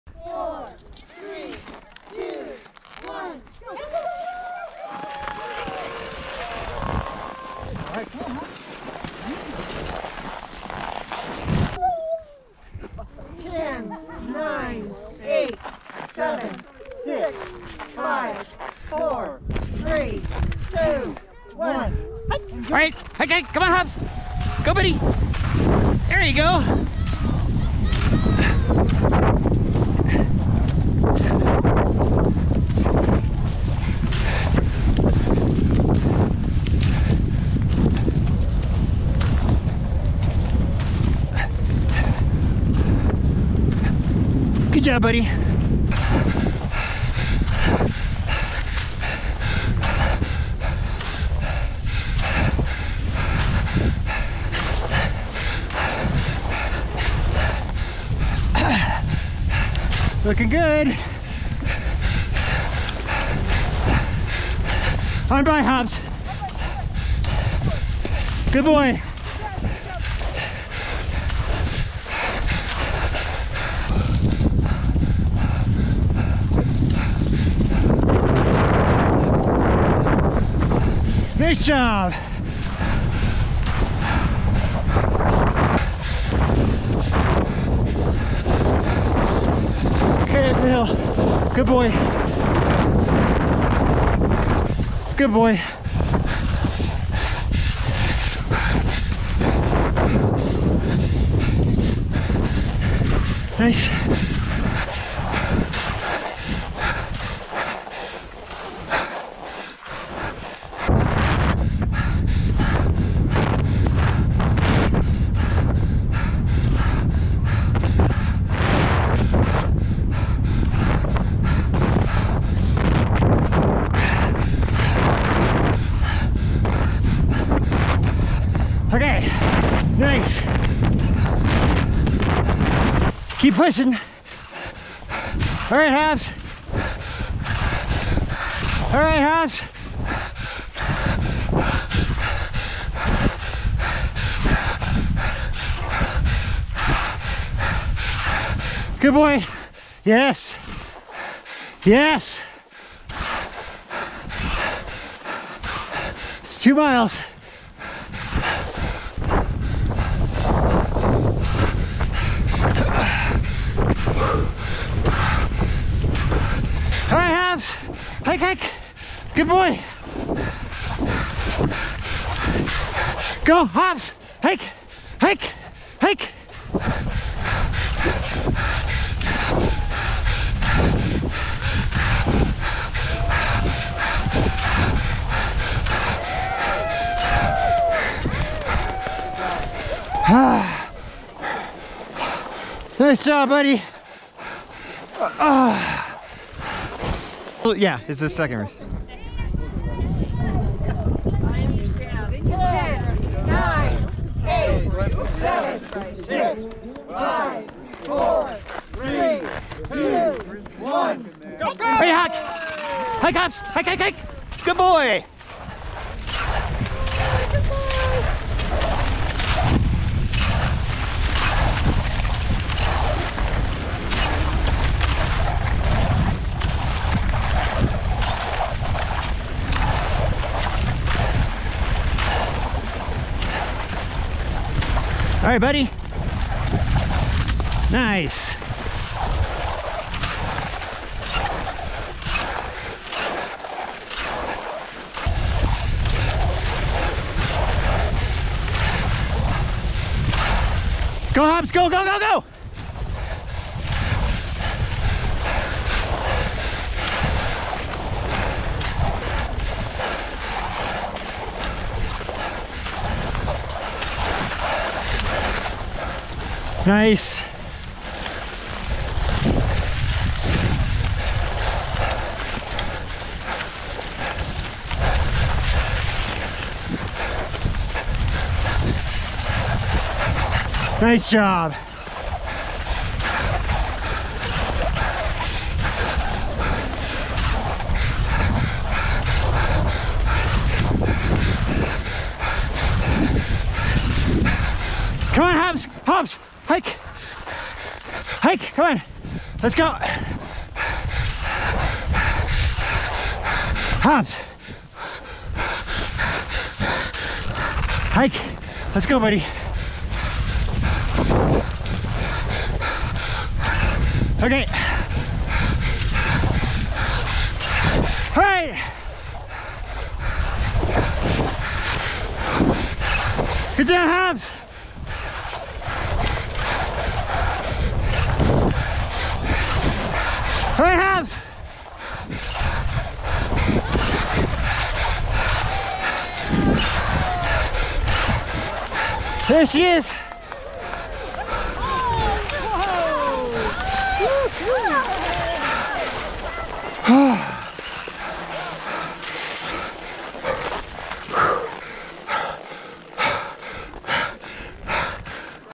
1-Dog Skijor at Grand Mesa Summit Challenge Sled Dog Race
Hobbes and I won the 1-dog skijor race at Grand Mesa with the fastest combined time over 2 days. Here's a little recap of both days of the race. Day 1 had a lot of fluffy blown snow and was a little slow, while day 2 had a harder, faster track! Hobbes had a little trouble when he saw Beth on the side of the track each day! He just wanted to stop to give her a lick. The race is held by the Rocky Mountain Sled Dog Club rmsdc.org